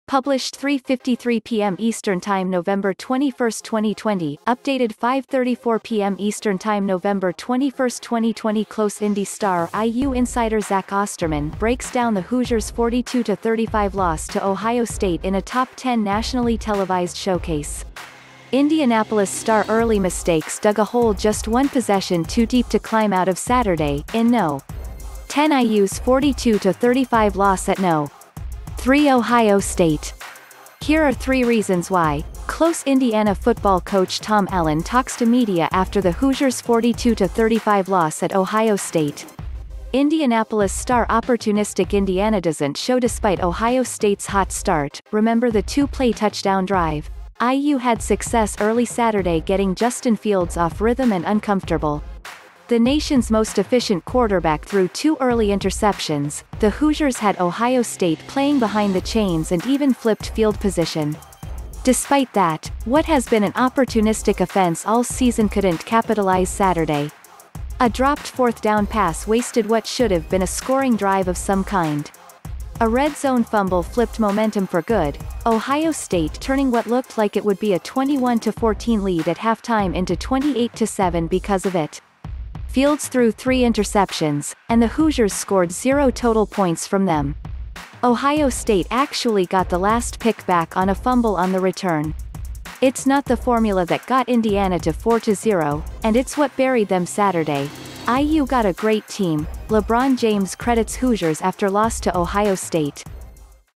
IU football rally falls short in loss to Ohio State but Hoosiers show
Published 3:53 p.m. ET Nov. 21, 2020 | Updated 5:34 p.m. ET Nov. 21, 2020CLOSEIndyStar IU Insider Zach Osterman breaks down the Hoosiers' 42-35 loss to Ohio State in a top-10 nationally-televised showcase. Indianapolis StarEarly mistakes dug a hole just one possession too deep to climb out of Saturday, in No. 10 IU's 42-35 loss at No. 3 Ohio State. Here are three reasons why:CLOSEIndiana football coach Tom Allen talks to media after the Hoosiers' 42-35 loss at Ohio State. Indianapolis StarOpportunistic Indiana doesn’t showDespite Ohio State’s hot start (remember the two-play touchdown drive?), IU had success early Saturday getting Justin Fields off rhythm and uncomfortable. The nation’s most-efficient quarterback threw two early interceptions, the Hoosiers had Ohio State playing behind the chains and even flipped field position. Despite that, what has been an opportunistic offense all season couldn’t capitalize Saturday. A dropped fourth-down pass wasted what should’ve been a scoring drive of some kind. A red-zone fumble flipped momentum for good, Ohio State turning what looked like it would be a 21-14 lead at halftime into 28-7 because of it. Fields threw three interceptions, and the Hoosiers scored zero total points from them. Ohio State actually got the last pick back on a fumble on the return. It’s not the formula that got Indiana to 4-0, and it’s what buried them Saturday.'IU got a great team': LeBron James credits Hoosiers after loss to Ohio State